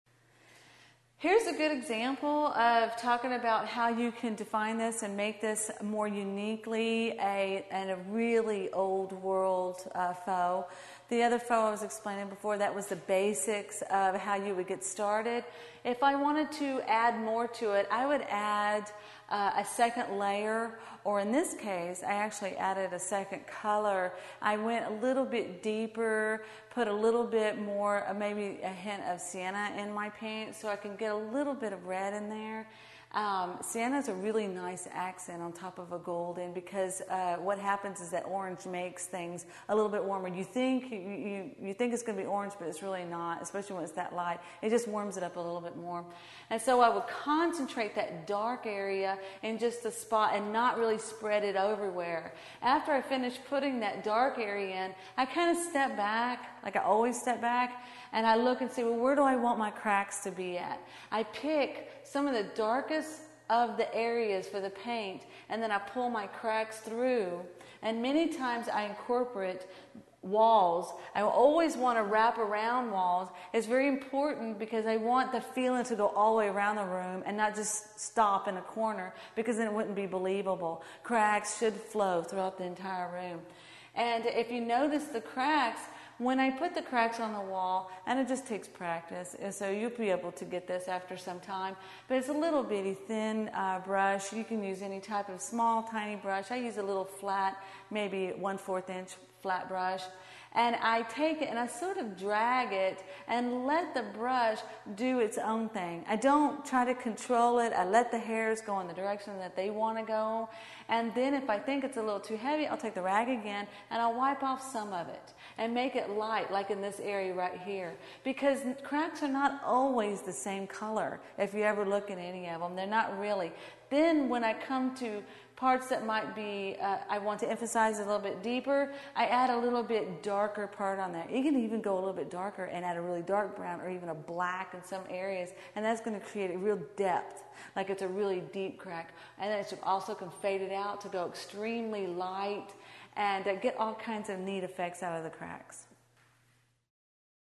Old World Faux Decorative Painting : Advanced Faux Painting Techniques
Old World Faux Decorative Painting : Advanced Faux Painting Techniques. Part of the series: How to Faux Paint. Learn an advanced technique to create a unique Old World faux look to your walls, in this free faux painting video.